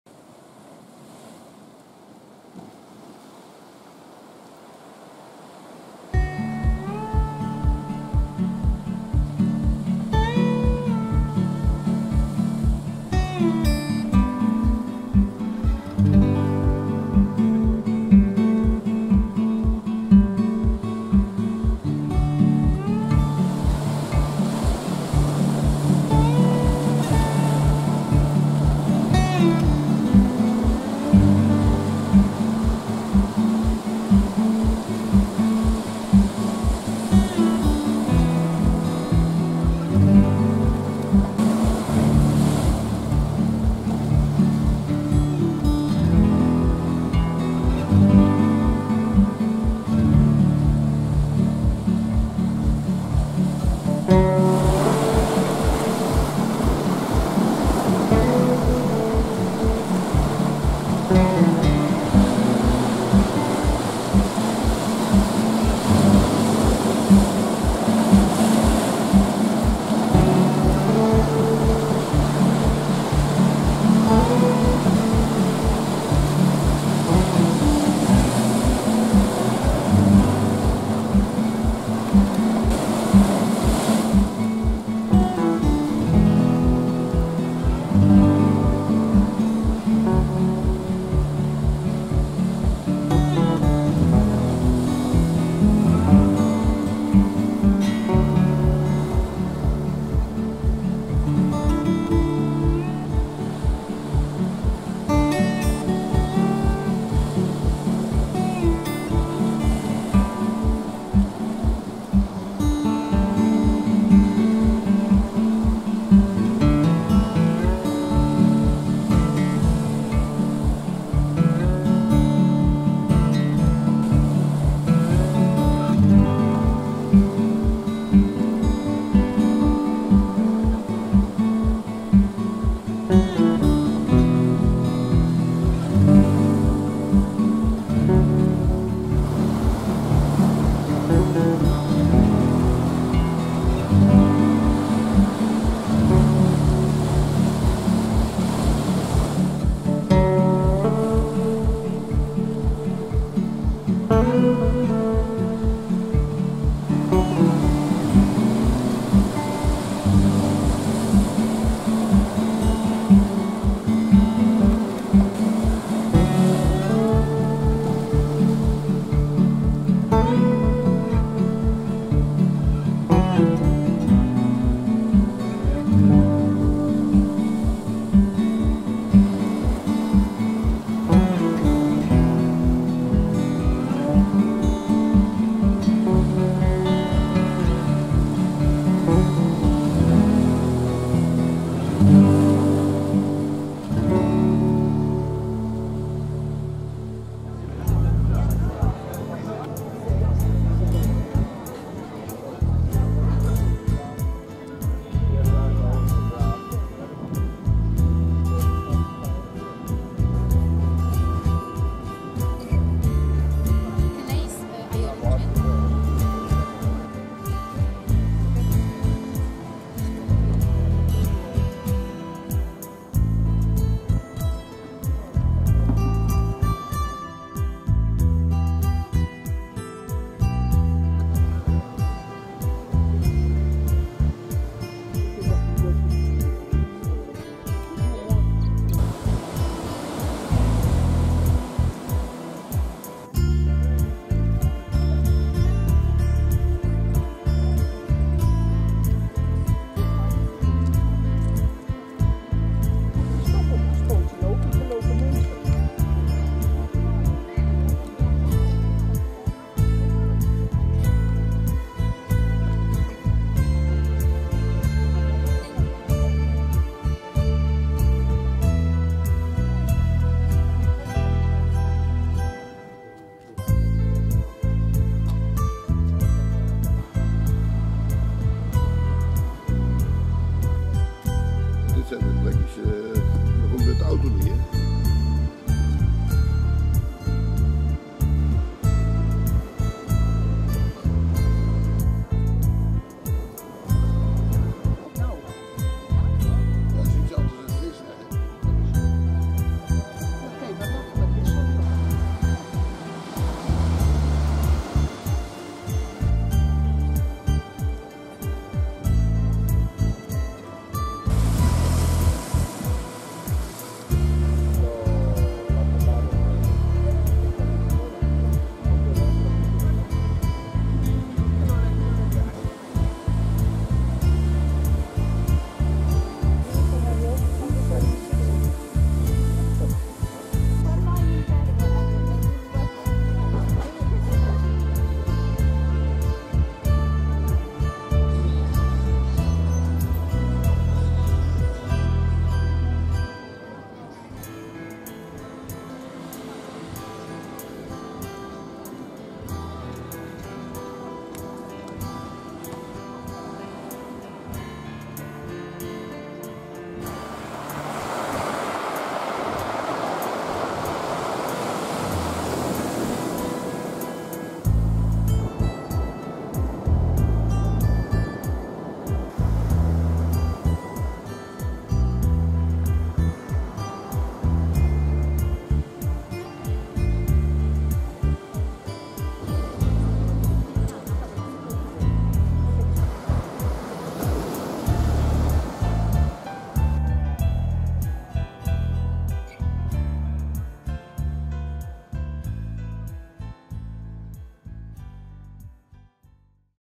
SPAIN Playa de las Americas, Tenerife (Canary Islands)
Playa de las Americas, together with Los Cristianos, is the most popular destination of Tenerife. There are many beaches and  the boulevards along the coast have a length of about 12 kilometers. There are many bars and restaurants with moderate prices.
(re-upload because of music rights claim)